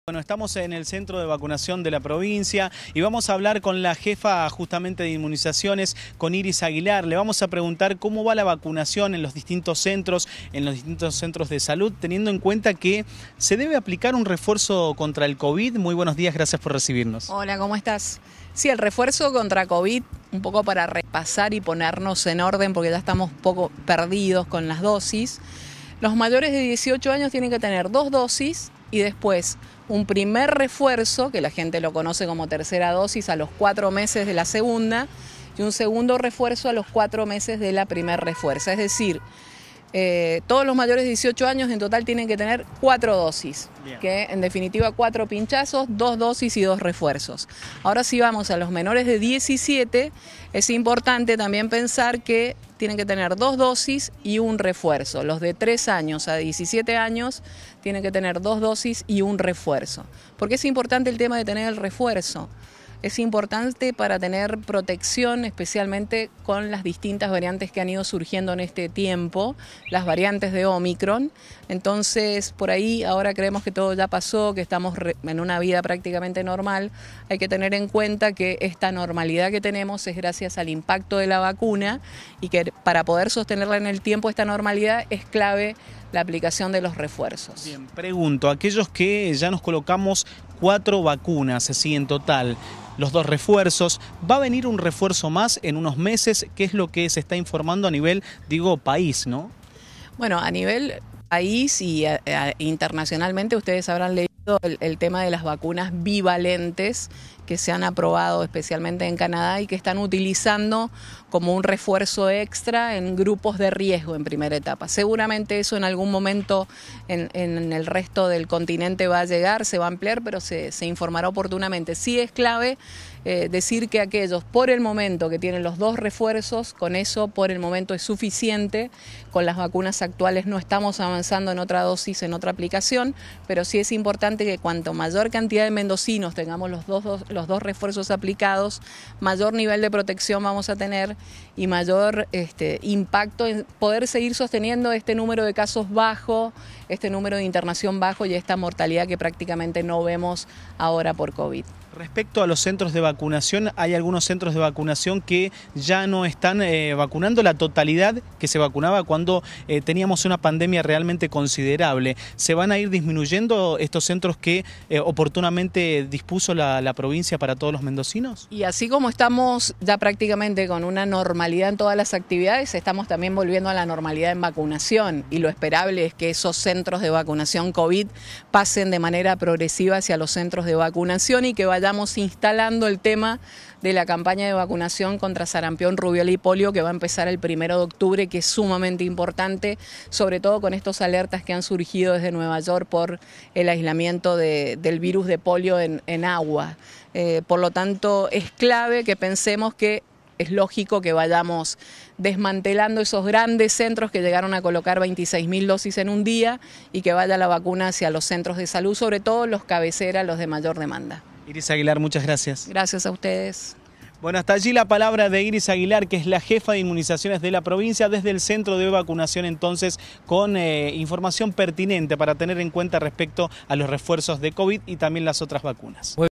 SIGUEN COLOCANDO REFUERZO COVID I El 1 de octubre empieza la campaña de sarampión, rubéola y polio
Iris Aguilar, jefe de Inmunizaciones, repasó cuantas dosis de la vacuna Covid debe tener colocada la población, según la edad. Y destacó que "esta normalidad que tenemos es gracias al impacto de la vacuna y es clave la colocación de los refuerzo".

La campaña de vacunación de sarampión, rubéola y polio empieza el 1 de octubre